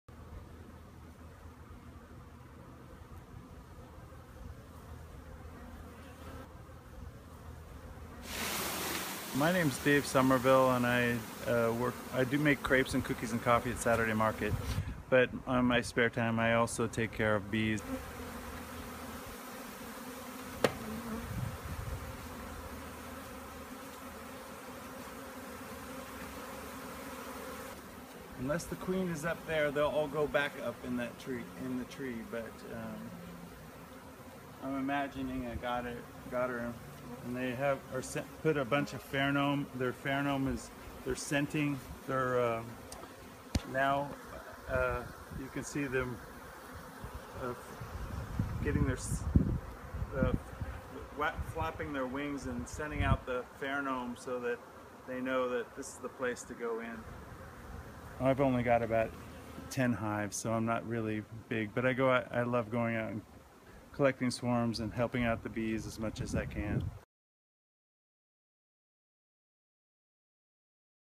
Capturing a spring swarm
Part-time beekeeper Dave Sommerville came to the rescue at a north Eugene residence recently after a swarm of honeybees clustered on a low-hanging magnolia tree branch. (Carl Davaz/The Register-Guard)